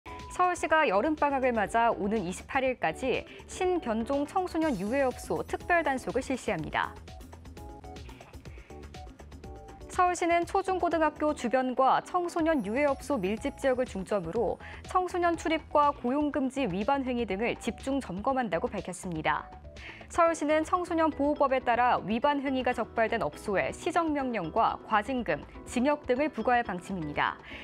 [소셜브리핑 - 크리스천을 위한 세상 보기] 사회ㅣ여름방학 맞아 신·변종 청소년 유해업소 특별단속 (이한승) l CTS뉴스
서울시가 여름방학을 맞아 오는 28일까지 신·변종 청소년 유해업소 특별 단속을 실시합니다.

시는 초·중·고등학교 주변과 청소년 유해업소 밀집 지역을 중점으로 청소년 출입과 고용 금지 위반행위 등을 집중 점검한다고 밝혔습니다.

서울시는 청소년 보호법에 따라 위반행위가 적발된 업소에 시정명령과 과징금, 징역 등을 부과할 방침입니다.